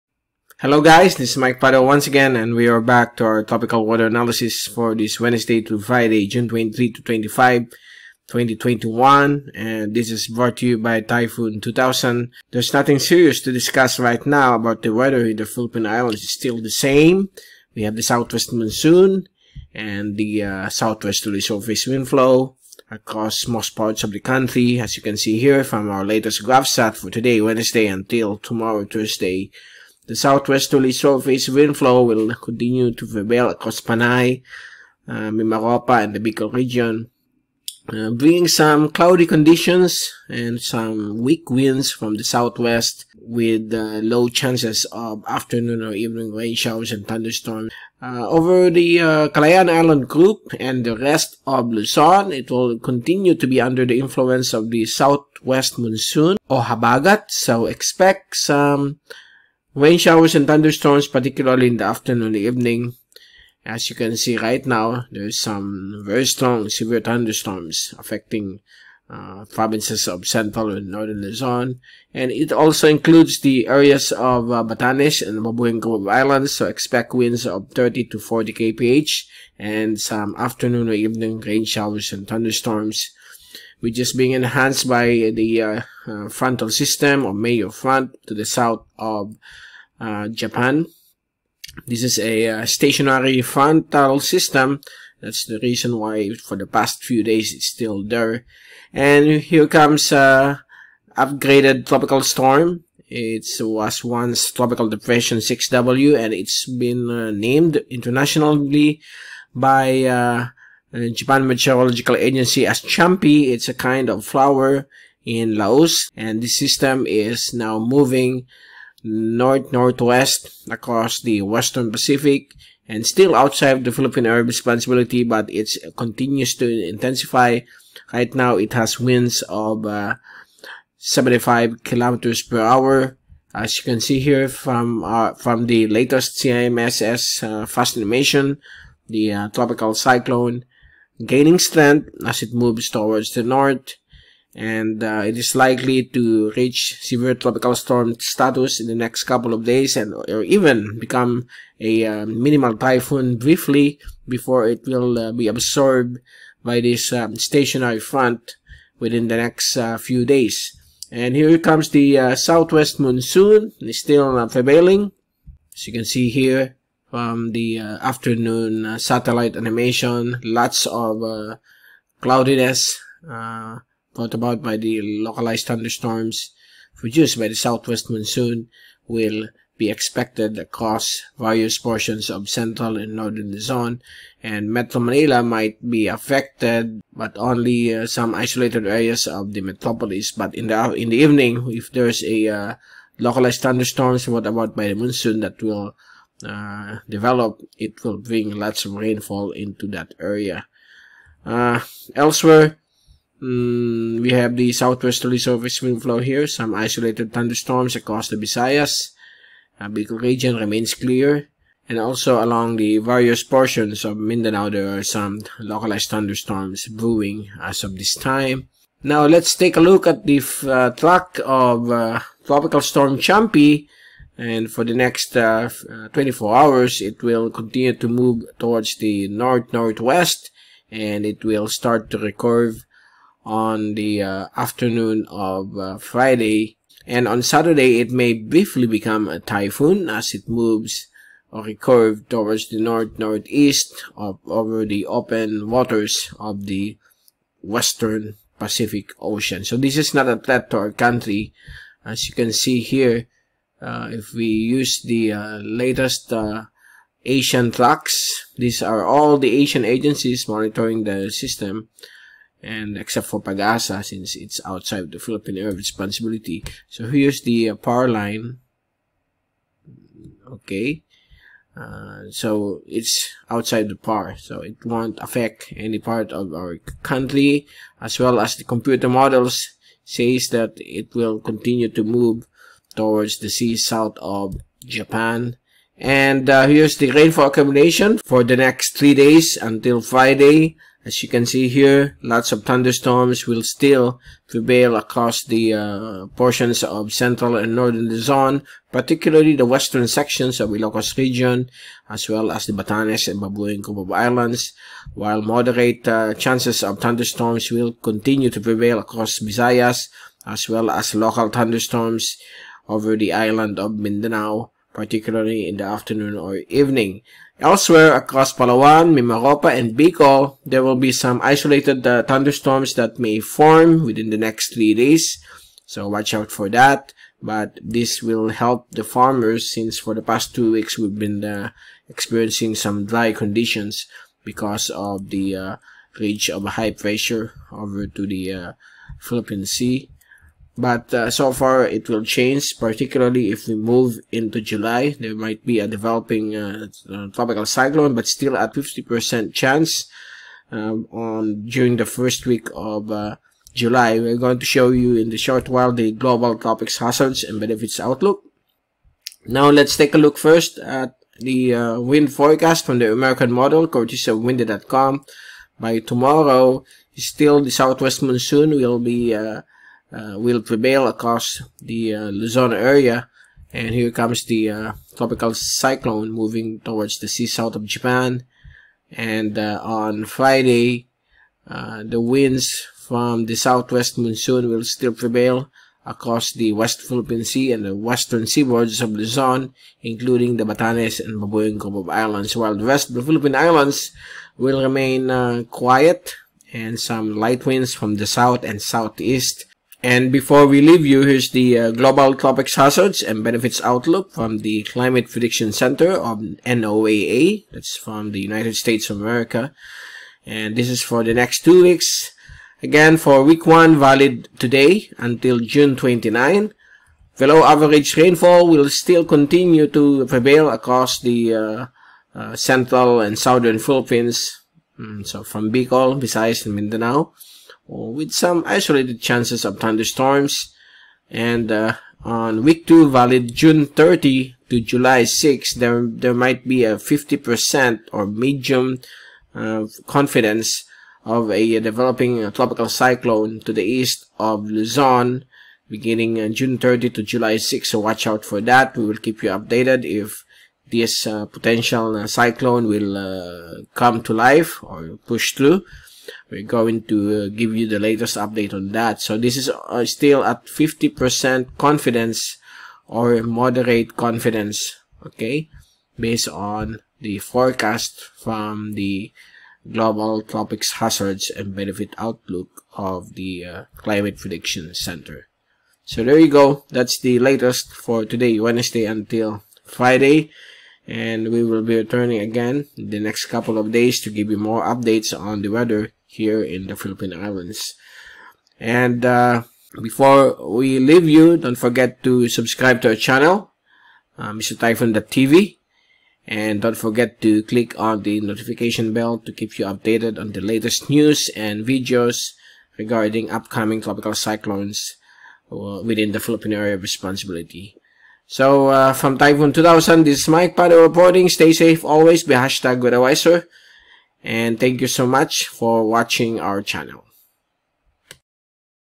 T2K Tropical Weather Analysis for June 23-25, 2021 (Wed-Fri) [English Ver]
Southwest Monsoon (Habagat) still affecting Northern & Central Luzon incl. Batanes & Babuyan Island Group where isolated to scattered rain showers & thunderstorms especially the western sections will be expected. Meanwhile, TD 06W has been upgraded to Tropical Storm (TS) now named as “Champi” & has began to track northward across the Western Pacific Ocean. This cyclone is not a threat to the Philippine Islands & will not enter the Philippine Area of Responsibility (PAR). To find out more, check out our T2K Tropical Weather Analysis recorded at 5:00 PM Manila Time (09:00 GMT) by Mike “Mr. Typhoon” Padua, Typhoon Specialist of Typhoon2000.com.